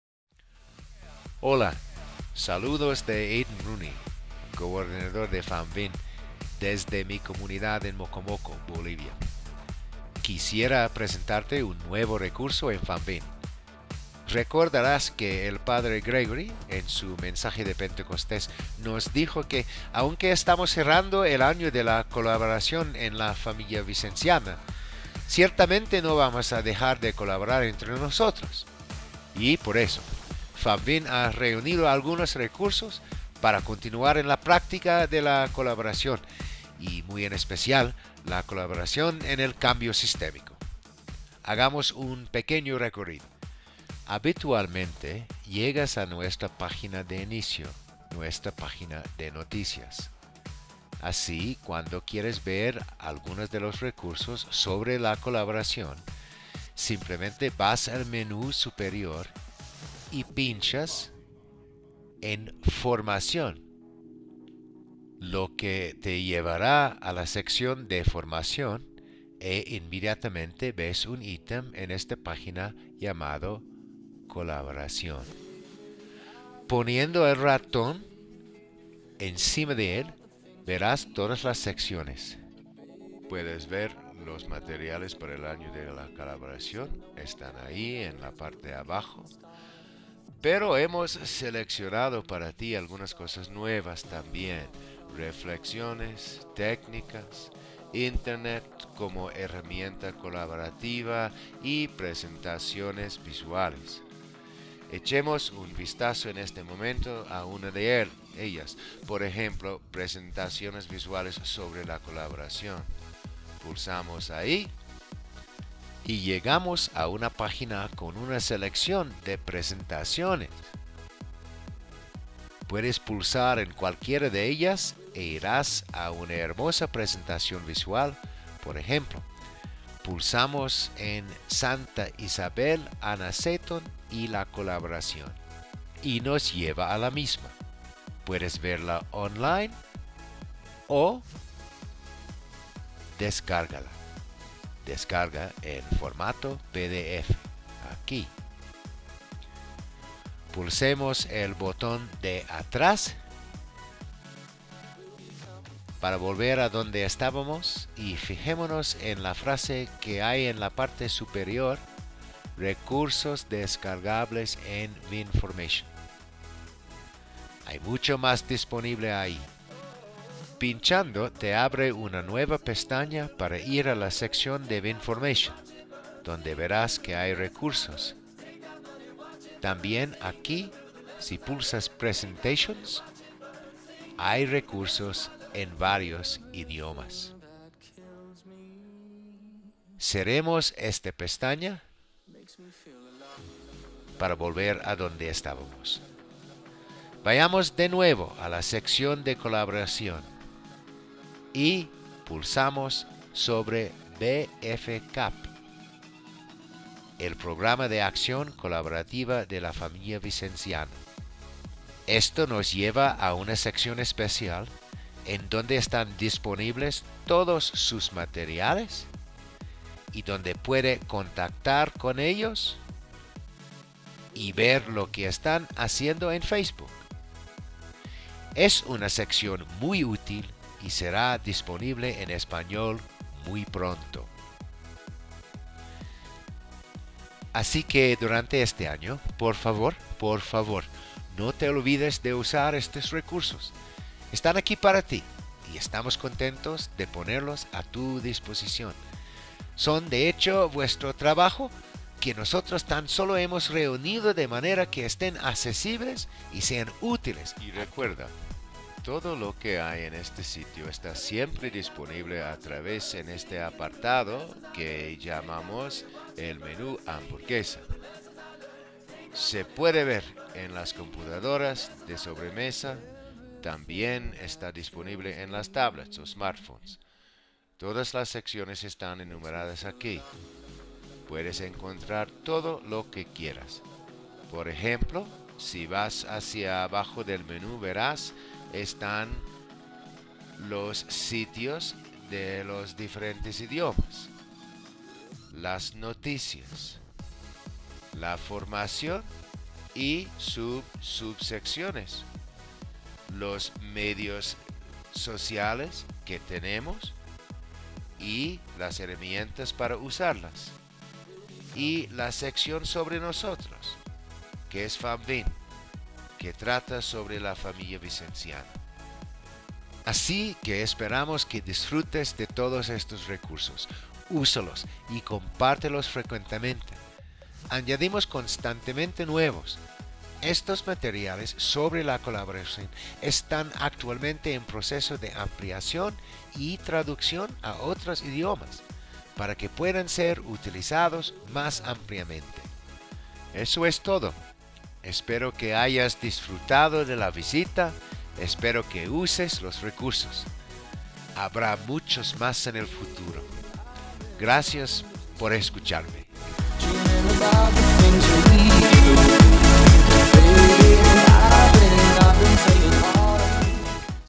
NUEVA EN .FAMVIN - sección sobre la colaboración
Para continuar manteniendo destacada la colaboración, .famvin ha creado una nueva sección dedicada a la colaboración. El P. Aidan R. Rooney, C.M., Coordinador Internacional de .famvin, presenta esta sección en el siguiente vídeo: